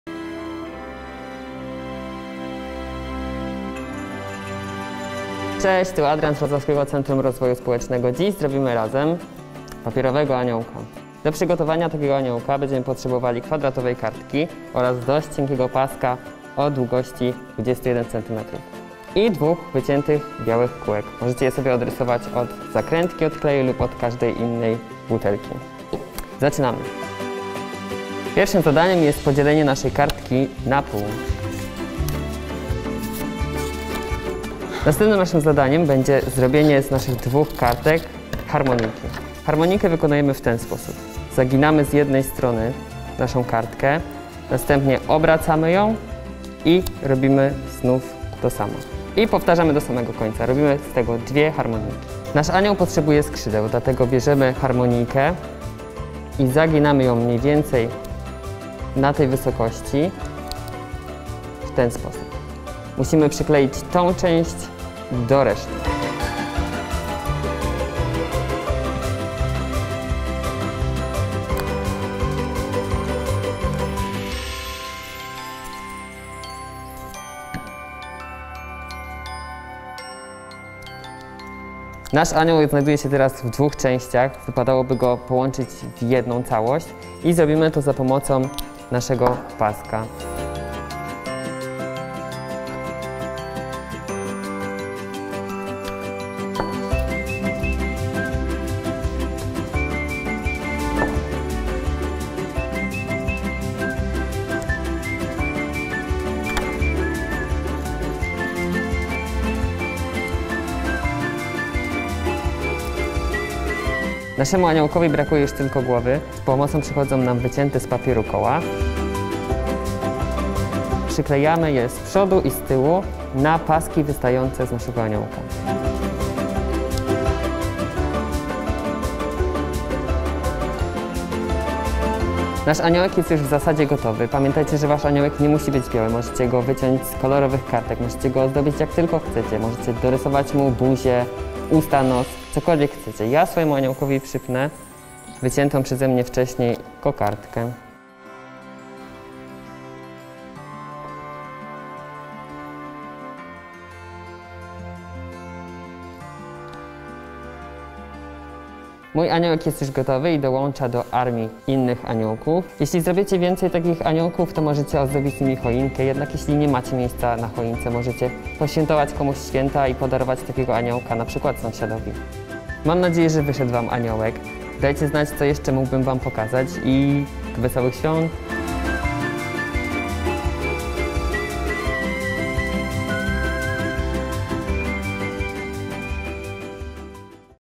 Anioł z papieru na choinkę. Poradnik krok po kroku
Chcecie zrobić papierową ozdobę na swoją choinkę? Razem z Wrocławskim Centrum Rozwoju Społęcznego przygotowaliśmy prosty poradnik krok po kroku, jak zrobić anioła z papieru. Zobaczie, jak stworzyć własnoręcznie taką ozdobę i i do dziełą! Anioł z papieru. Origami. Ozdoby na choinkę.
Zapraszamy też na stronę akcji WCRS Poświętuj komuś święta